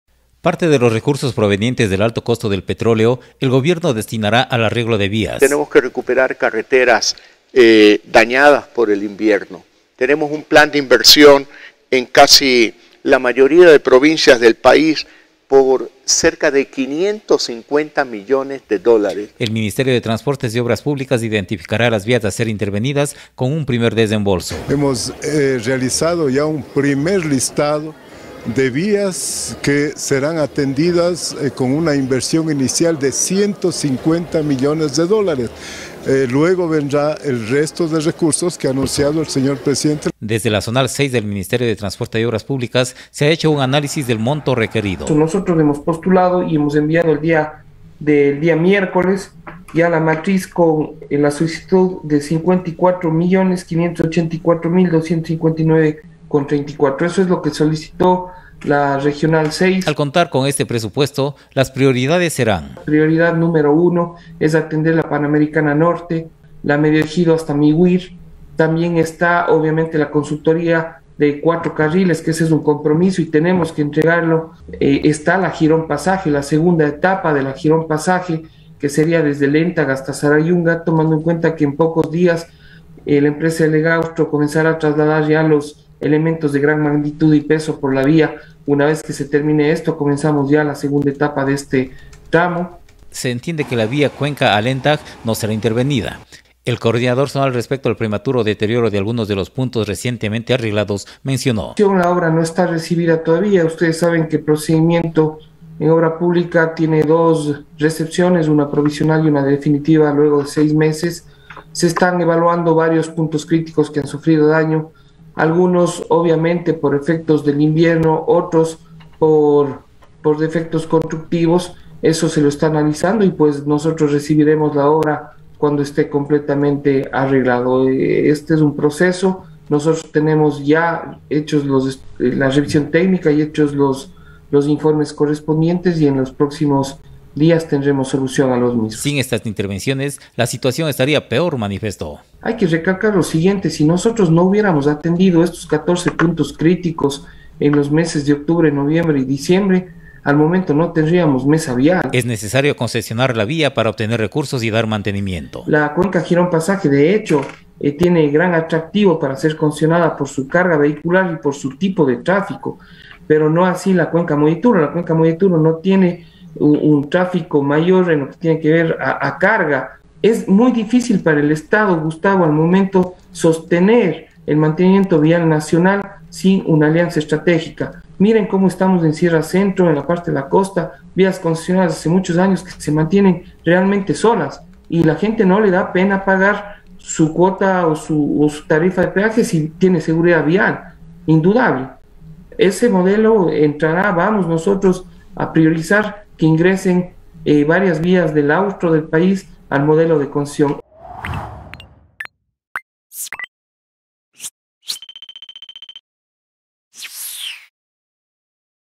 La vialidad estatal en la jurisdicción de Girón está en mal estado
La vía estatal que cruza por Girón está en mal estado a pesar de que hace pocas semanas mediante un convenio entre el MTOP y la empresa ASFALTAR EP se dio mantenimiento a 13 puntos críticos. La semana anterior el presidente de la república anunció la inversión de recursos provenientes del alto costo del petróleo para el mantenimiento vial del país; al respecto, el Coordinador Zonal 6 del MTOP, en radio Antena Uno, comentó el monto requerido en el austro y cuáles serán las vías a intervenir, no consta la jurisdicción de Girón.